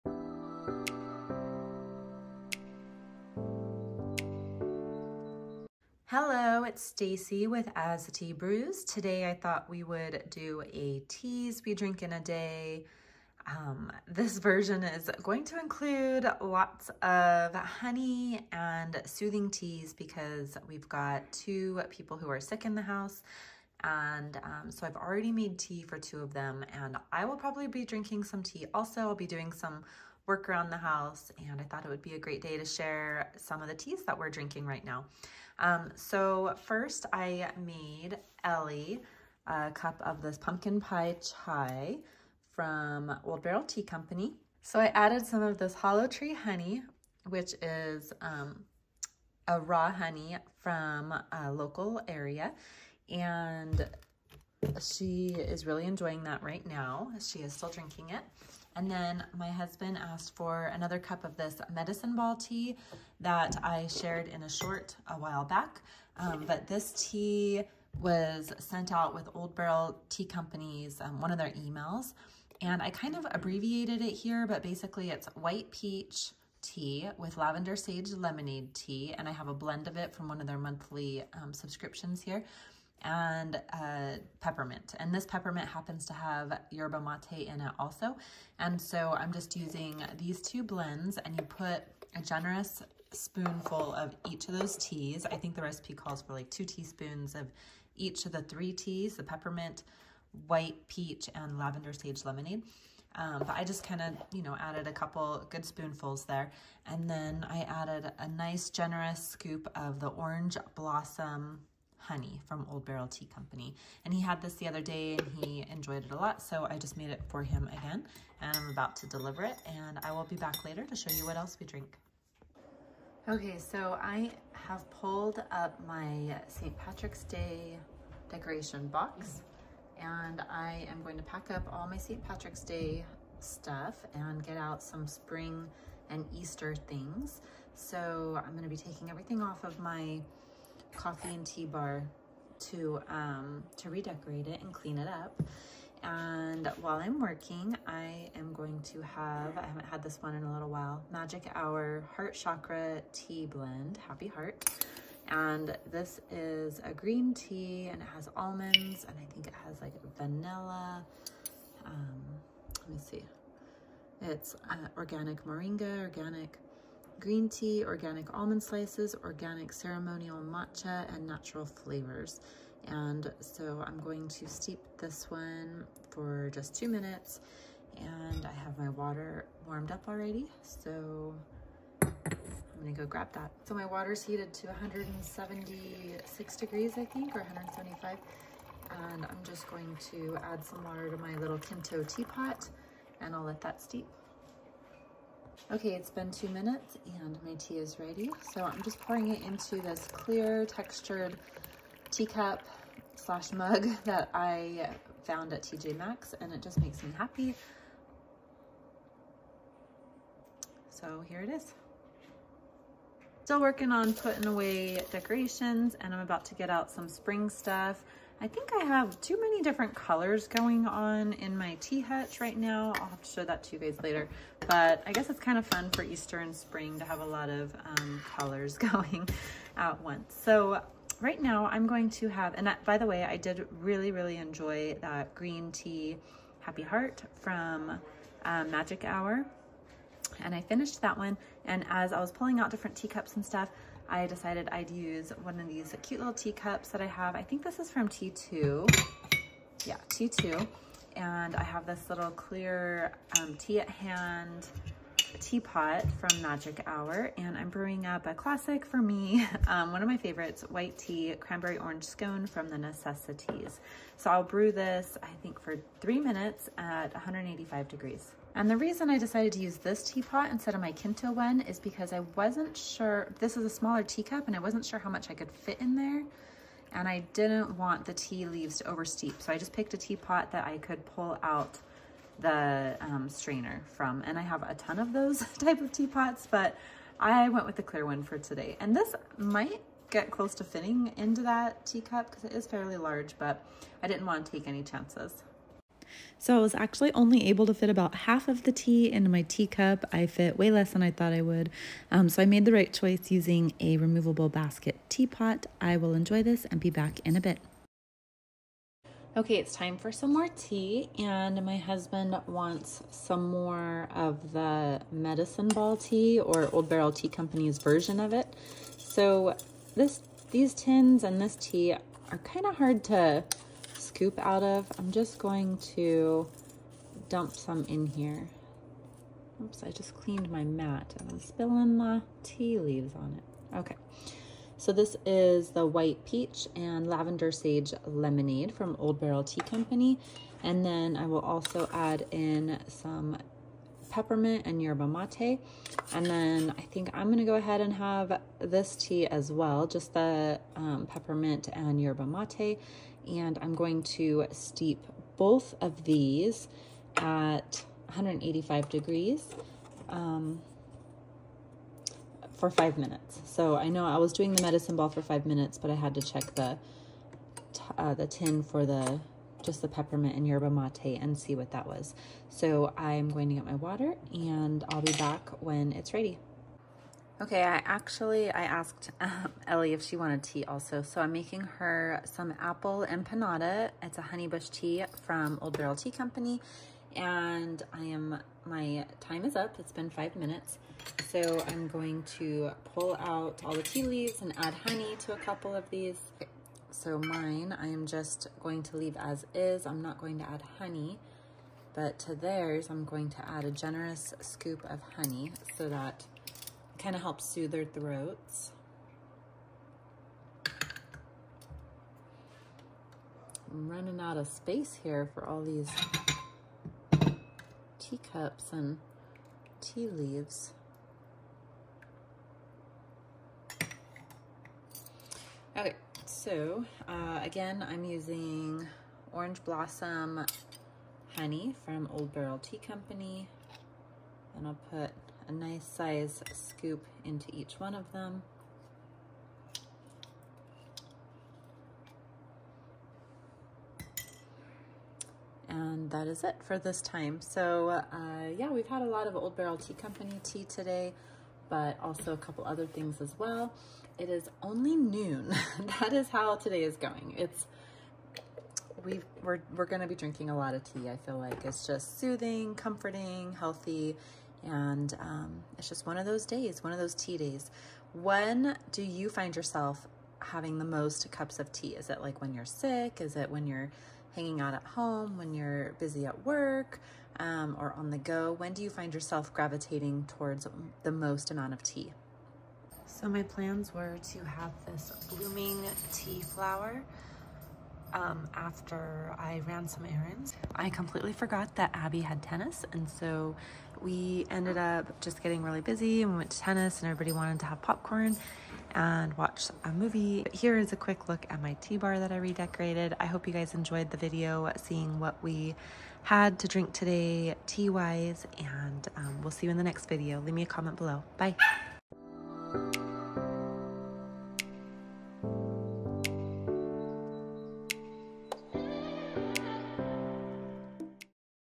Teas we drink on a sick day || plus medicine ball tea recipe!
Here’s a look at the teas we drank while two of our family members were sick! What teas do you drink when you’re sick?

My favorite teapot-
Teapot- Kinto Stainless Unitea One Touch Teapot 460 ml. Heat-resistant glass teapot with stainless steel strainer and lid.